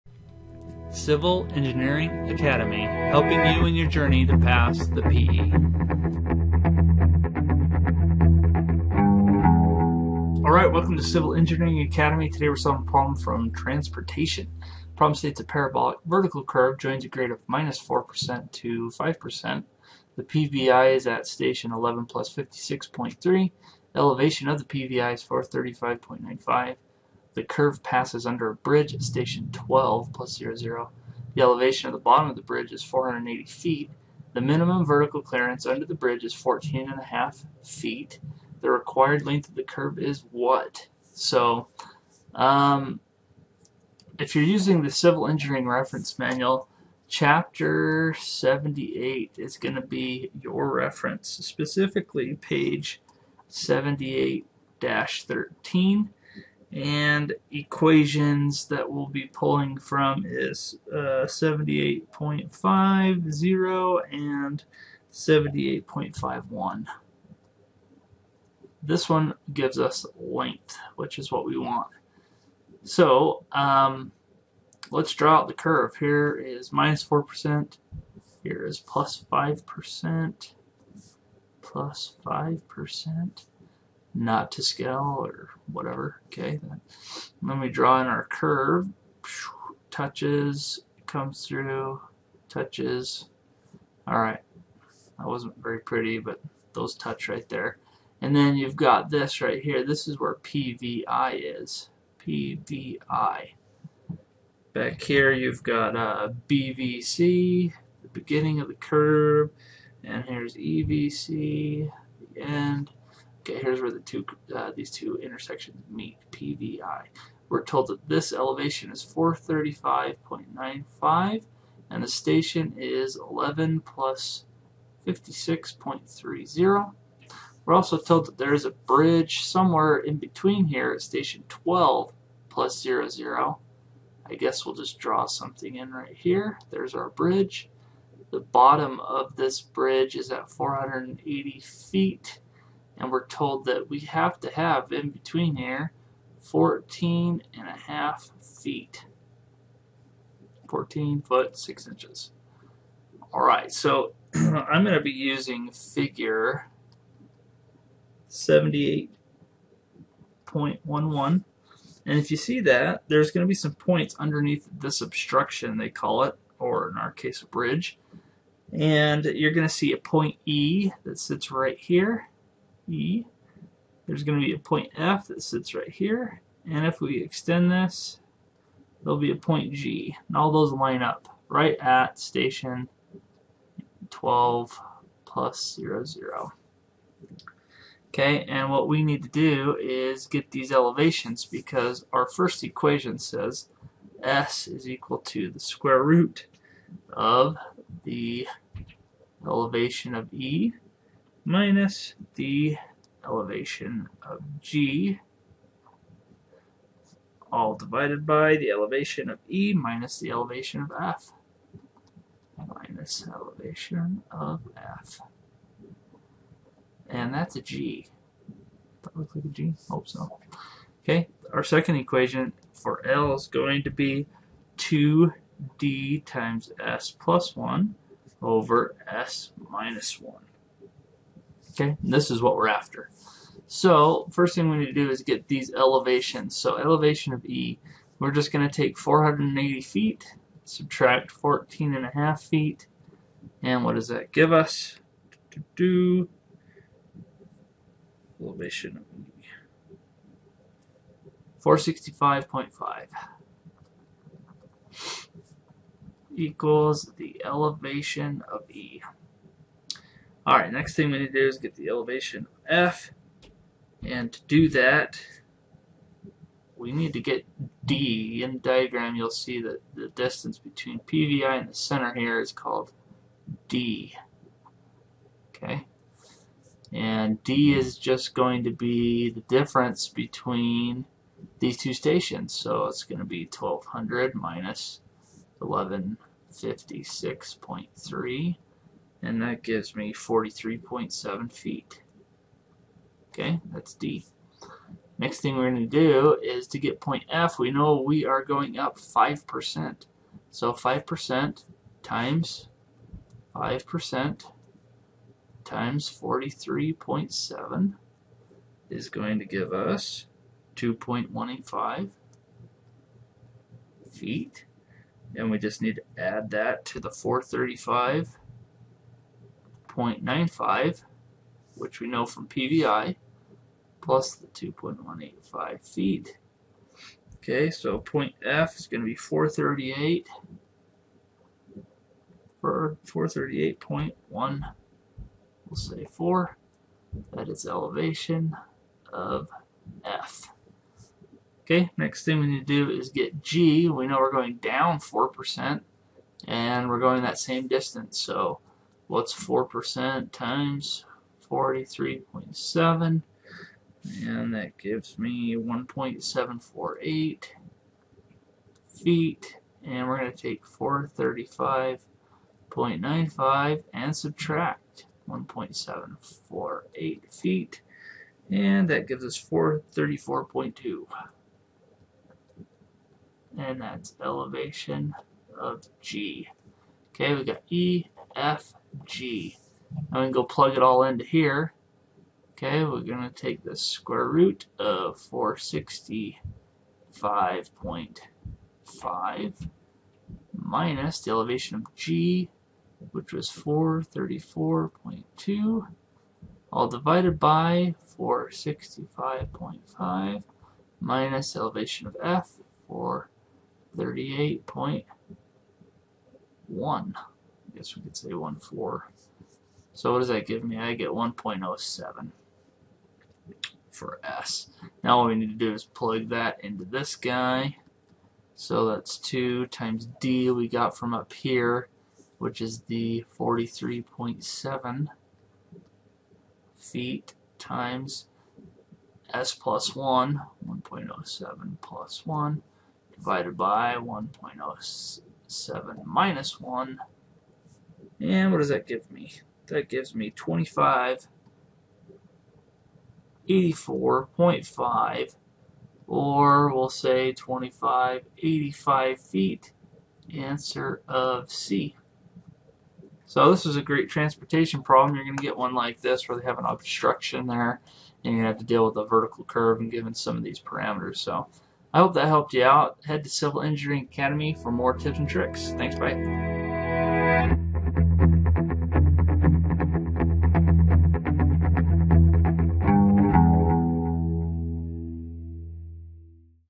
Transportation - Find the Length of Vertical Curve with Obstruction
Transportation problem covering the clearance under a bridge.  Great vertical curve problem for the civil PE exam.

If you need advice, more tips, great practice exams for the civil PE exam, solved problems, or courses, make sure to check out our home base: civliengineeringacademy.com